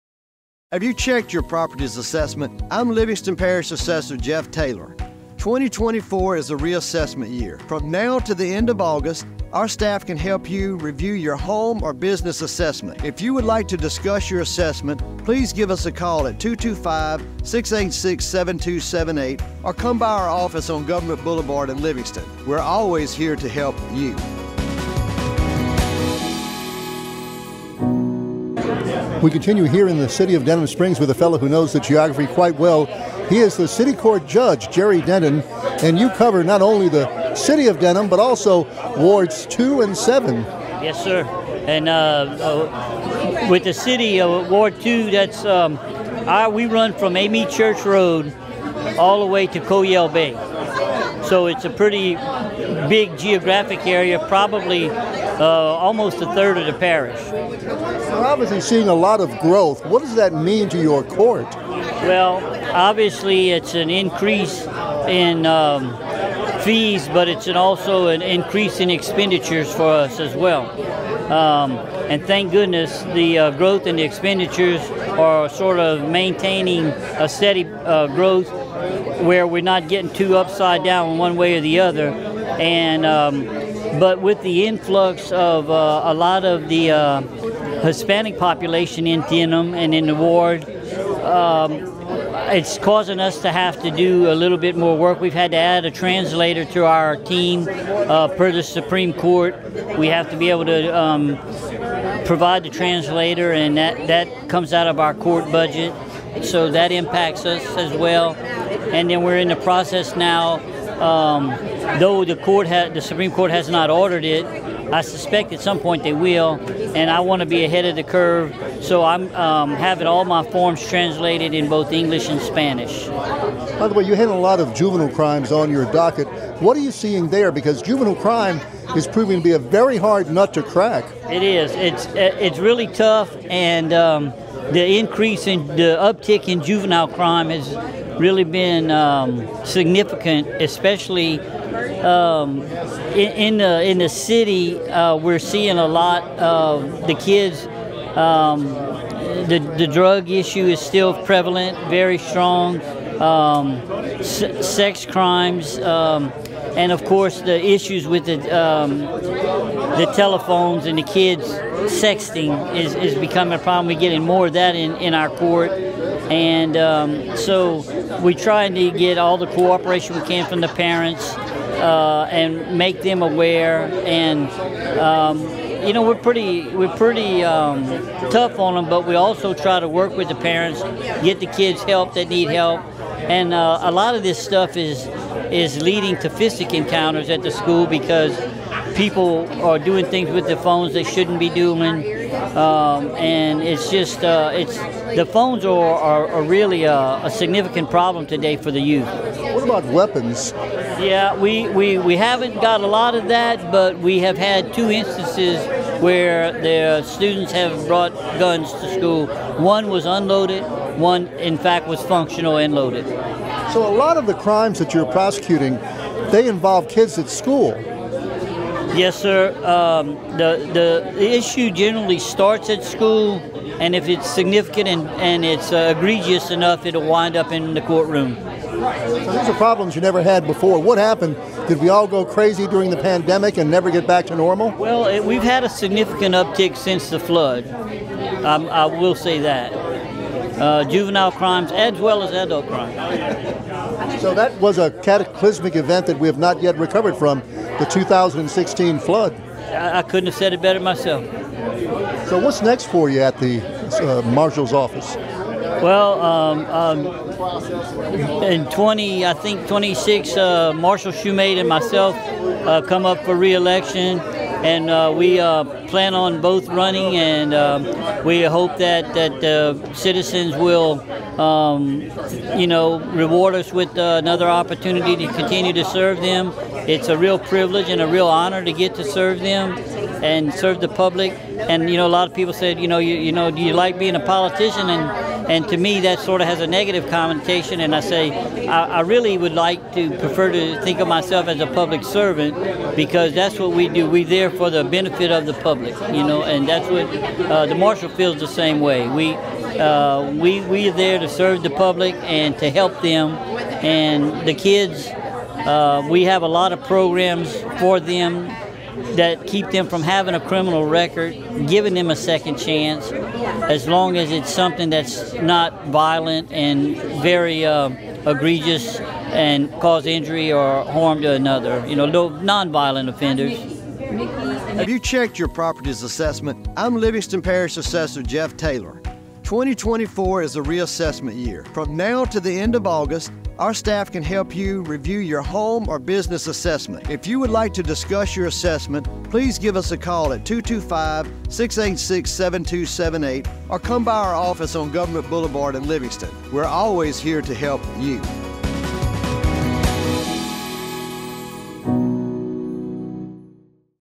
Jerry Denton
Denham Springs City Court Judge Jerry Denton sees the impacts of growth and the challenges of juvenile crime.